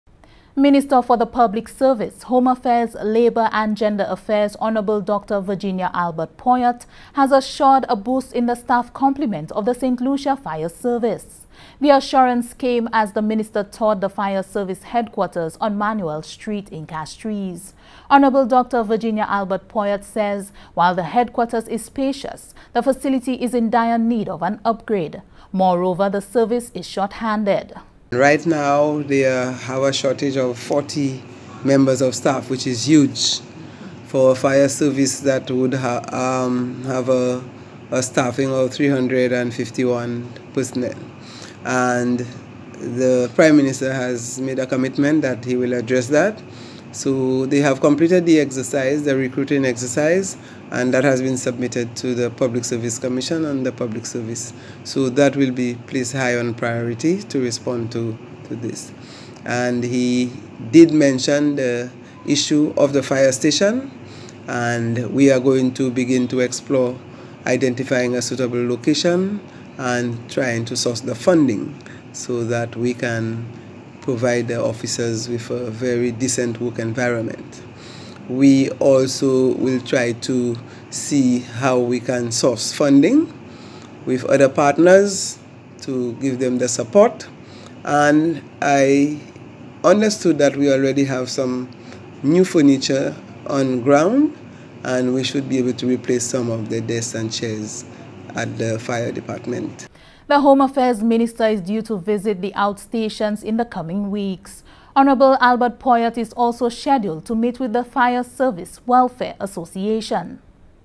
Government to increase Fire Service staff complement
Minister for the Public Service, Home Affairs, Labour and Gender Affairs, Hon. Dr. Virginia Albert Poyotte has assured a boost in the staff compliment of the Saint Lucia Fire Service. The assurance came as the minister toured the Fire Service Headquarters on Manoel Street in Castries. Hon. Dr. Virginia Albert Poyotte, says while the headquarters is spacious, the facility is in dire need of an upgrade. Moreover, the service is shorthanded.